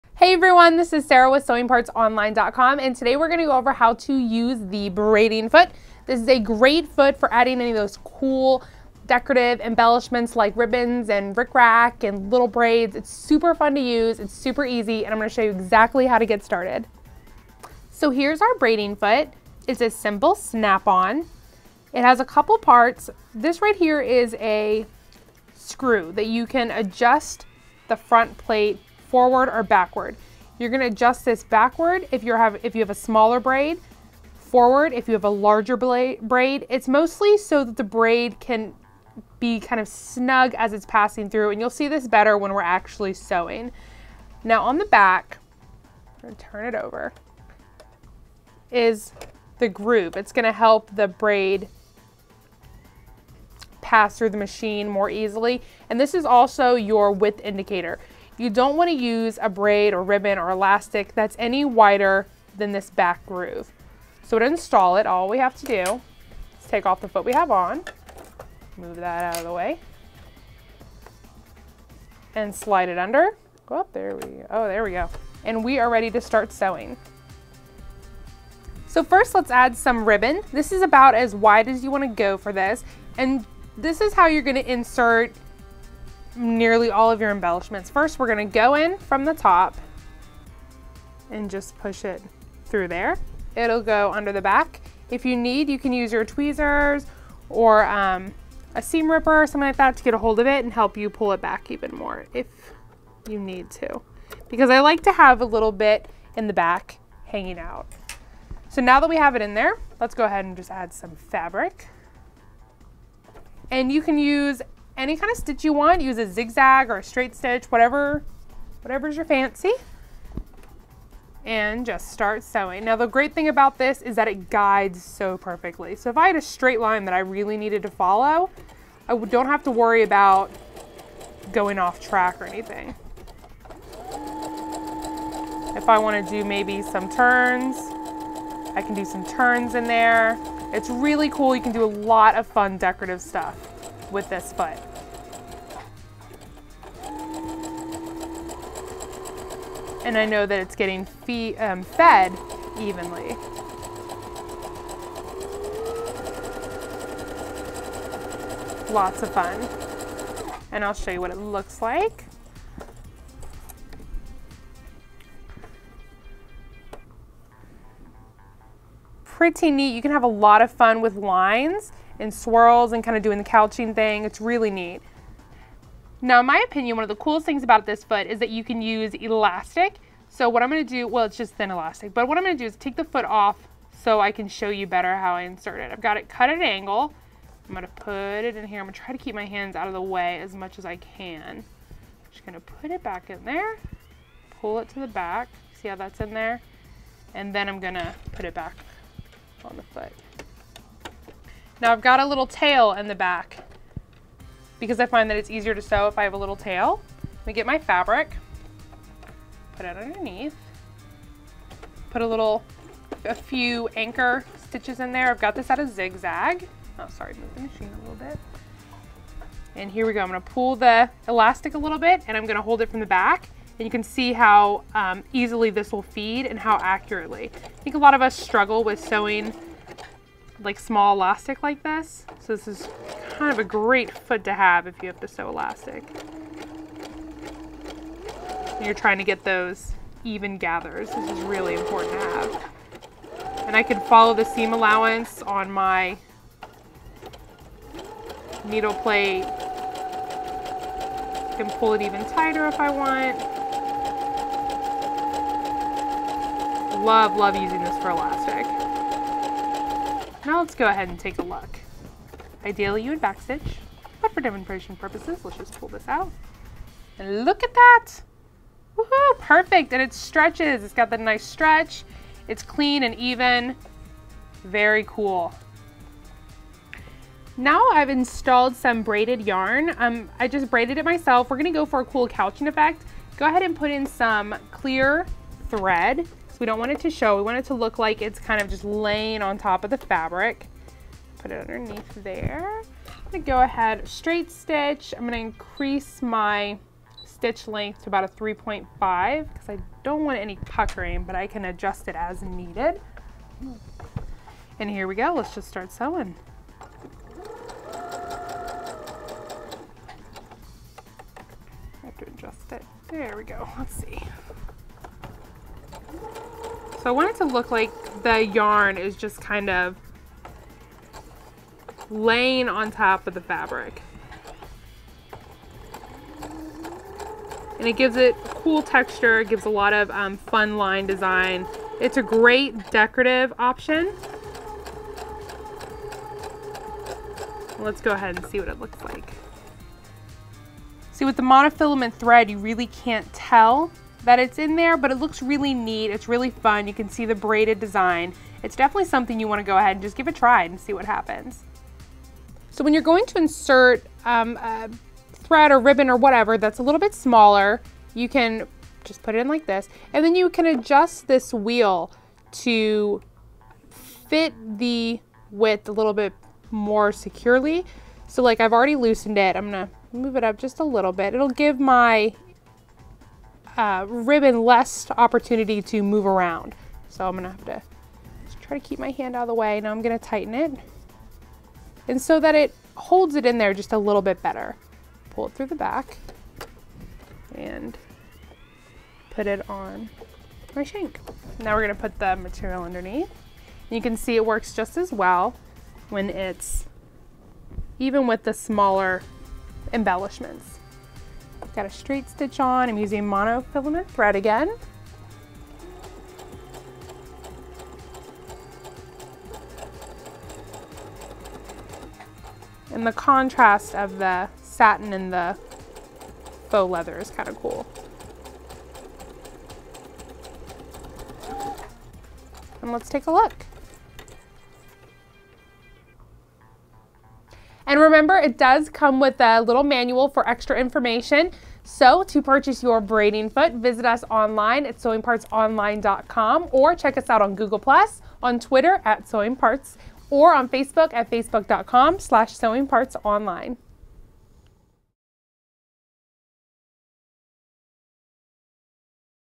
How To Use A Braiding Foot
The Braiding Foot is the easiest and fastest way to attach trim, yarn, ribbon, rick rack, and even elastic.  Trim and elastic 5mm wide or less is most ideal for this foot.  Simply insert your trim or elastic through the front opening, pull a few inches past the back of the foot, and start sewing. Adjust the front screw to fit your trim and prevent any mistakes or imperfections.

If you have any comments or questions, be sure to leave them in the comment section below and be sure to join our sewing community: